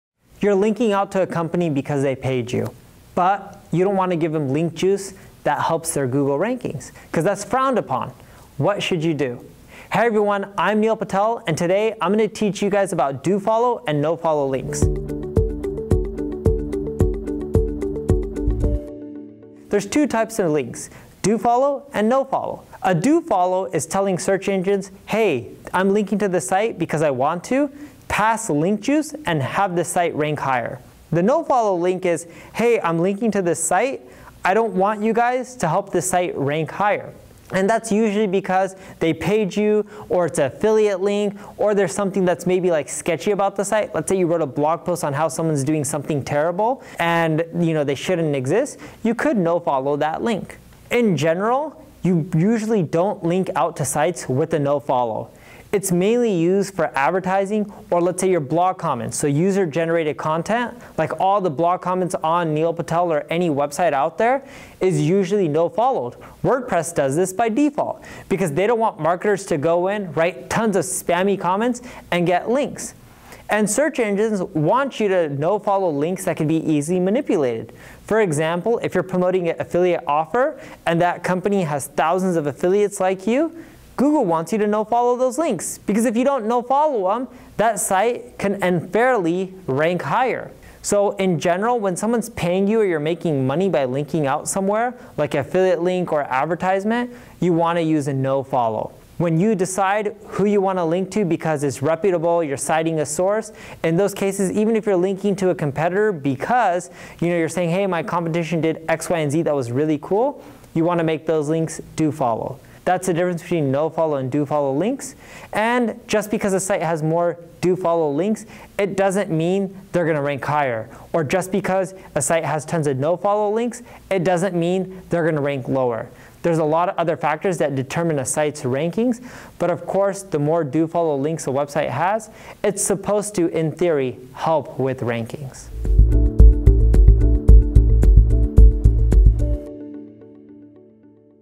What's The Difference Between Do Follow and No Follow Links
There are two types of links that you need to be familiar with: do-follow and no-follow links.

A "do-follow" link tells search engines that this link is good and that you don't mind if the links helps them rank higher.

A "no-follow" link tells a search engine that the link isn't approved by you or that it is paid and that the link shouldn't help the site rank higher.

If you sell backlinks and "do-follow" them, you can potentially get penalized.

And if your website contains user generated content, such as message board or blog comments, no-follow the links.

When you are citing a source, even if it is a competitor, you should do-follow the link.

Now you know the difference between those two link types and when you should use them.